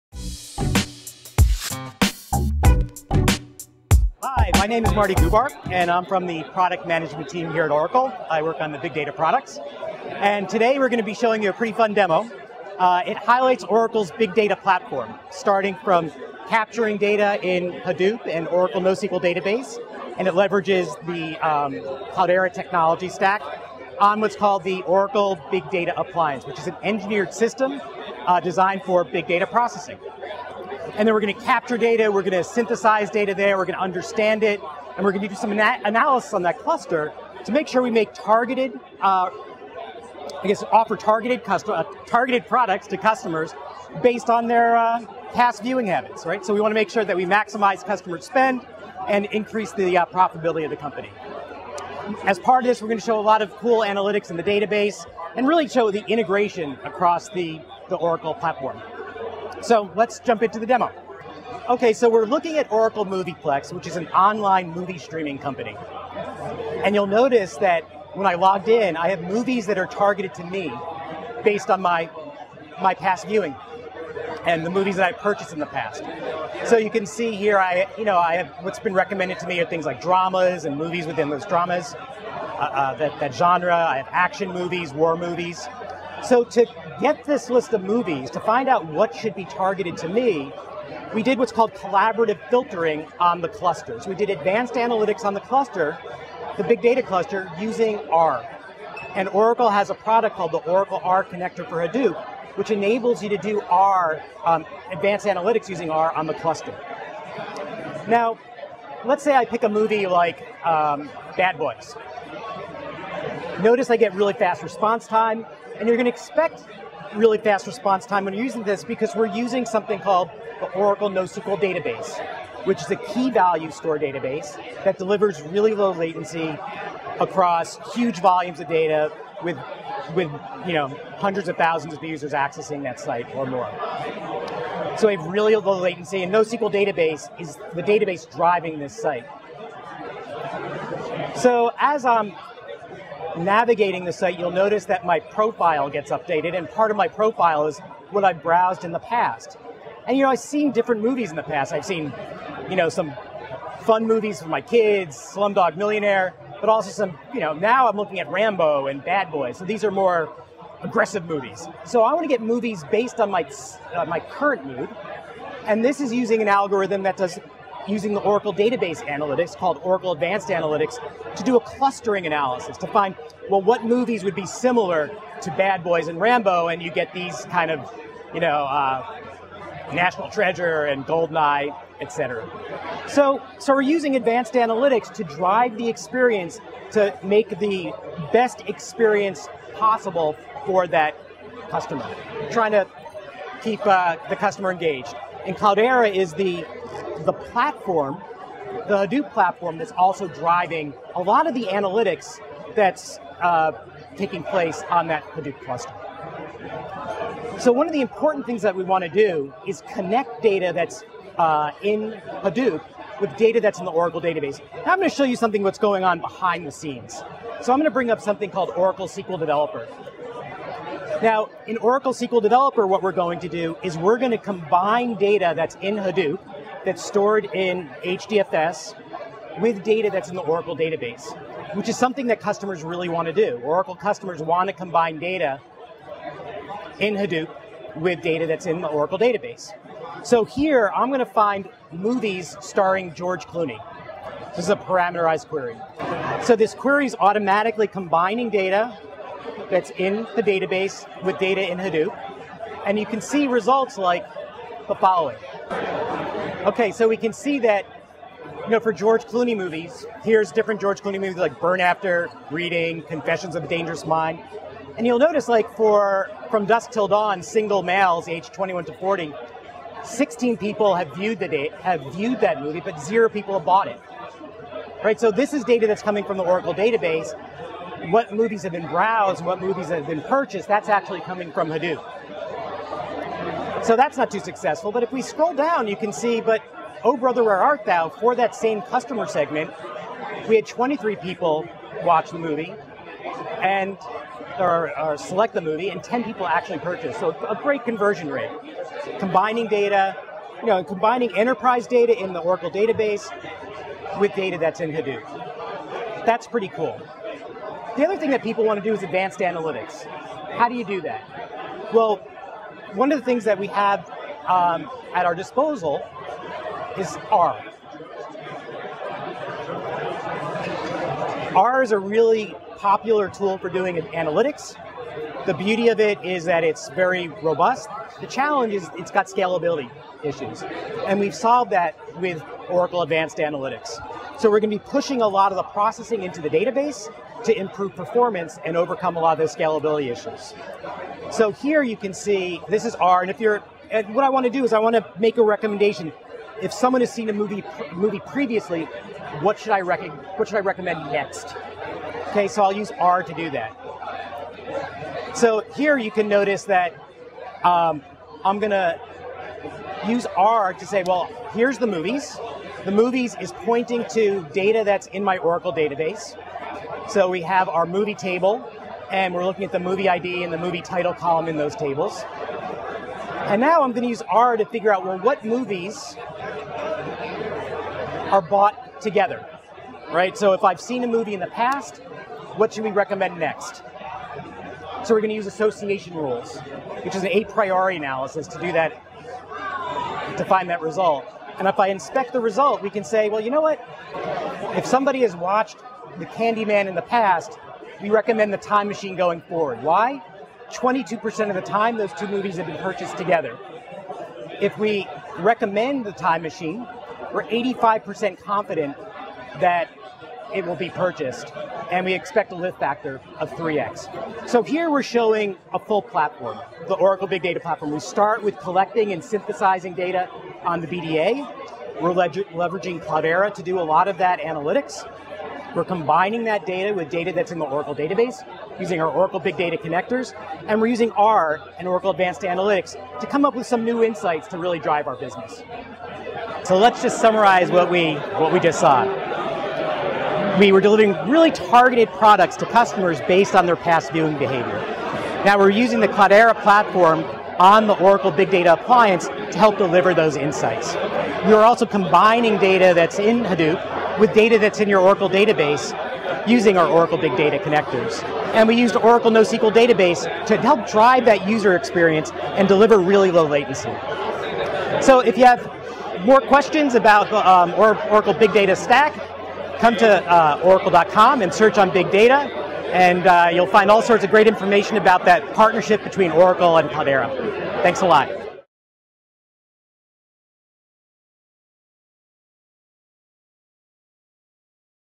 Cloudera and Oracle
A display of The Oracle Big Data Appliance, an engineered system optimized for acquiring, organizing and loading unstructured data into Oracle Database 11g. The Oracle Big Data Appliance includes CDH, Oracle NoSQL Database, Oracle Data Integrator with Application Adapter for Apache Hadoop, Oracle Loader for Hadoop, an open source distribution of R, Oracle Linux, and Oracle Java HotSpot Virtual Machine.